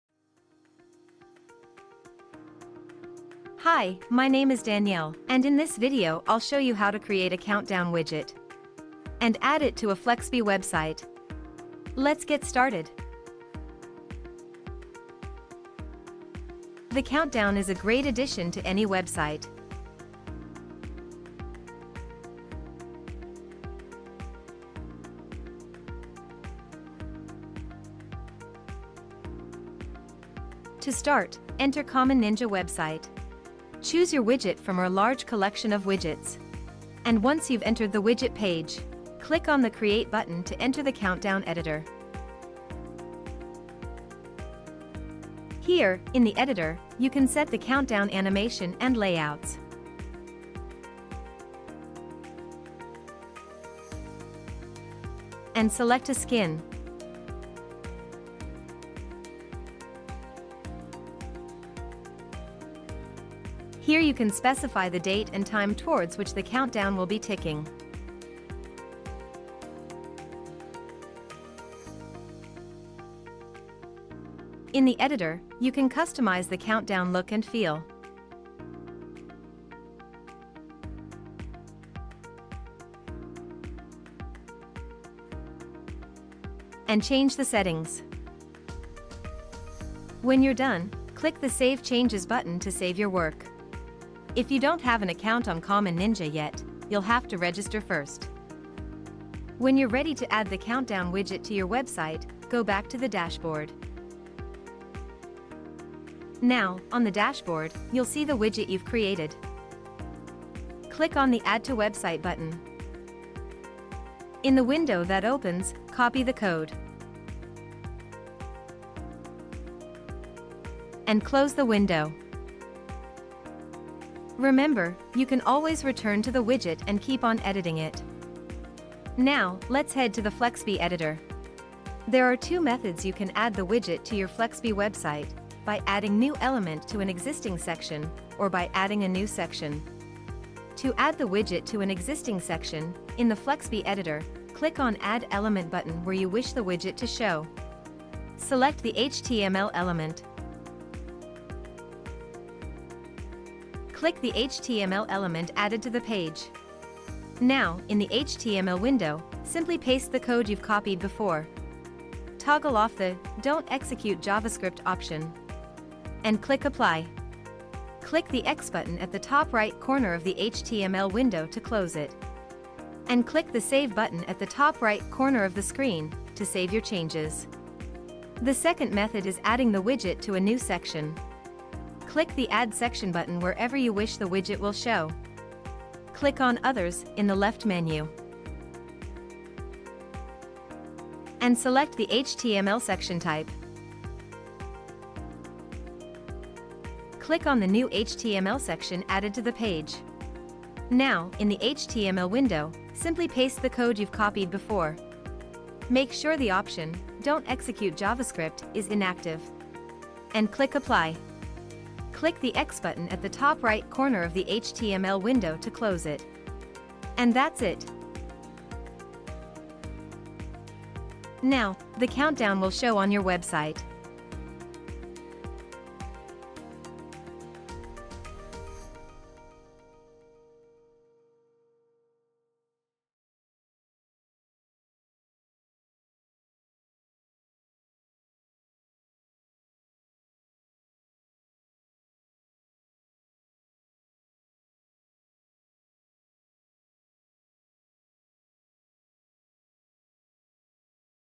How to add a Countdown to Flexbe
Common Ninja’s Countdown widget is an eye-catching, visually appealing tool that can grab attention and encourage users to perform an action on your website. With it, you can inform users of deals and discounts, encourage them to explore the website, and convert them by playing on their fear of missing out (FMO).

Video Timeline
00:00 About the Countdown widget
00:30 Creating a Countdown
00:44 Editing the Countdown
01:46 Adding the Countdown to Flexbe

The Countdown widget for Flexbe has a wide variety of customizable features, including:
- Localize titles and labels
- Beautiful layouts
- Festive confetti animation
- Fully customizable design